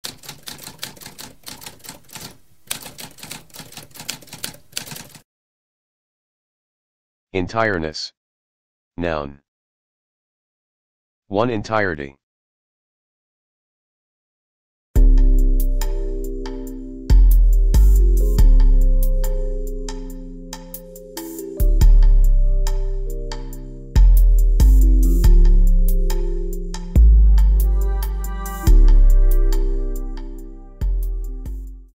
What does entireness mean?
A spoken definition of entireness.

Intro Sound:
Typewriter - Tamskp